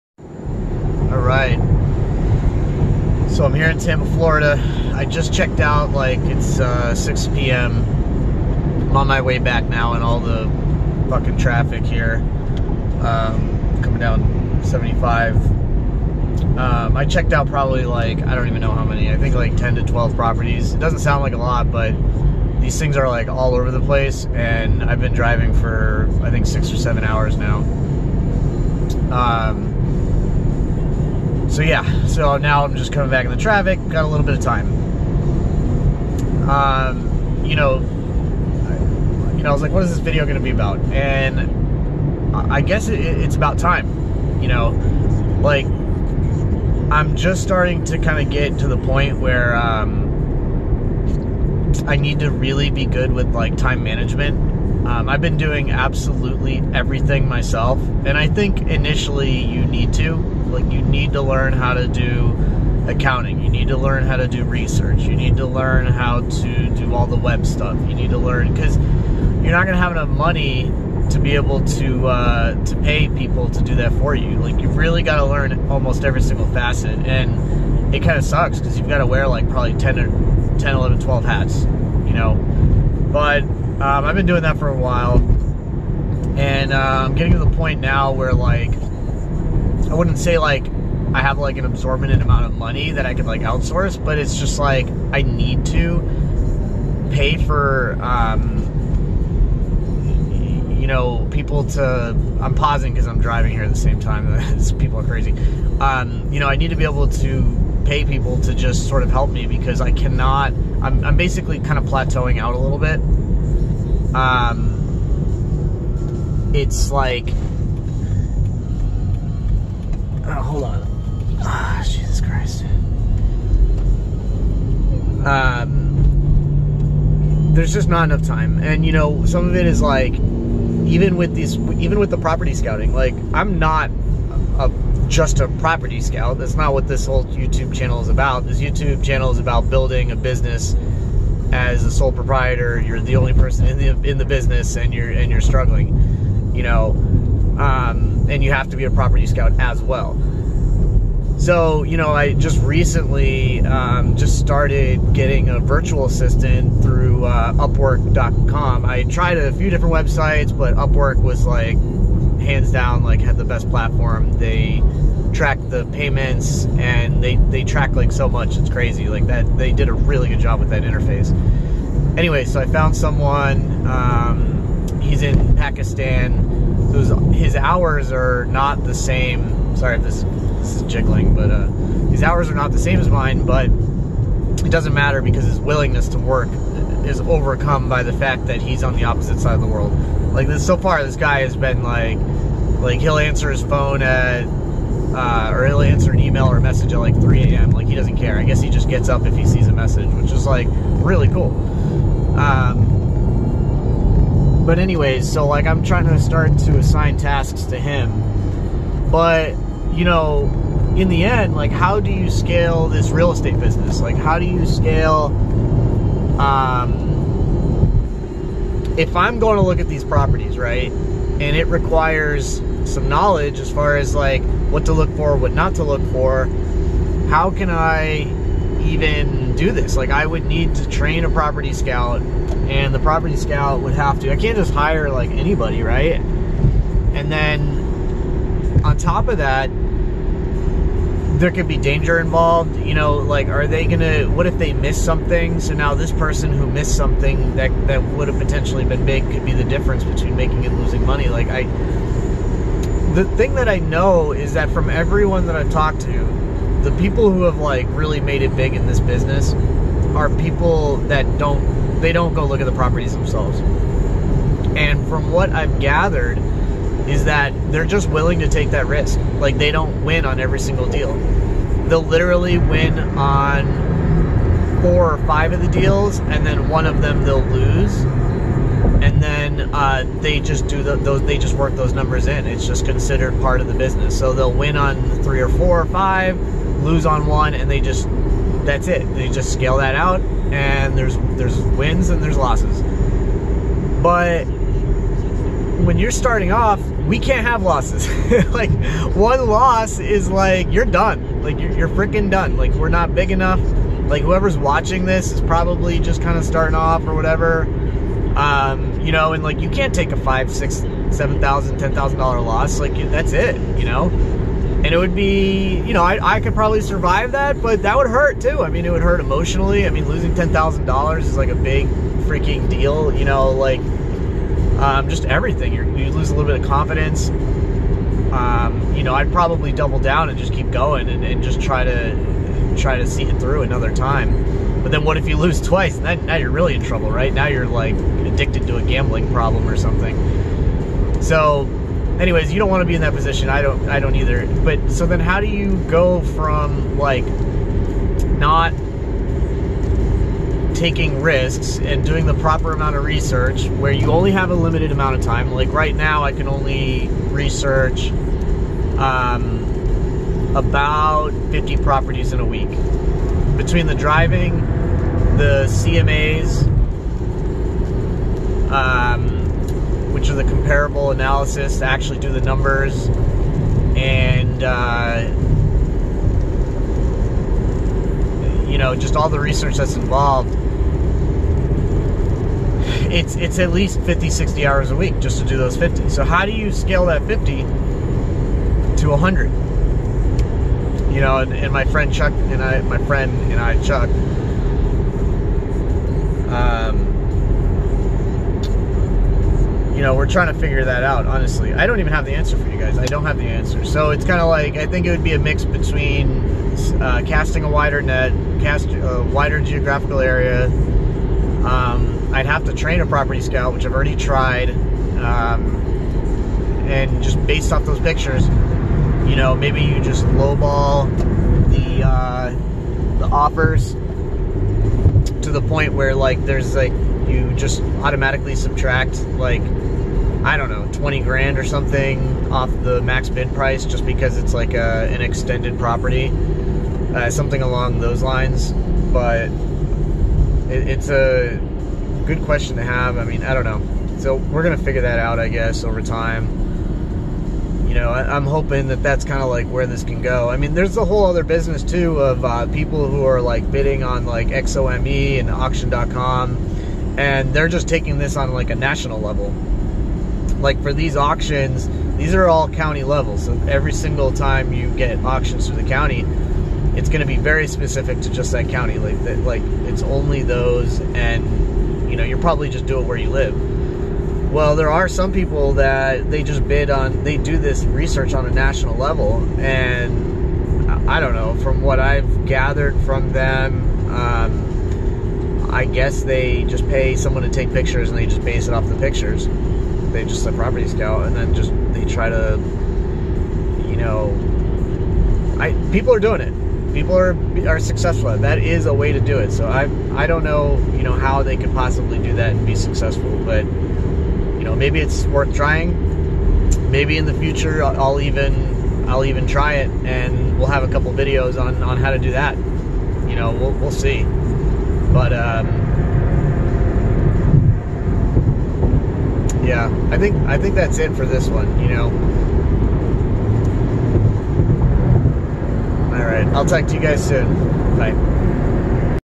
How to scale out your business with the property scouting
William Howell ads in a journal log about his experience today with the property scouting and different scenarios in which he can actually Outsource The Scouting to a property Scout and how he has been expanding his business with the use of the new virtual assistant.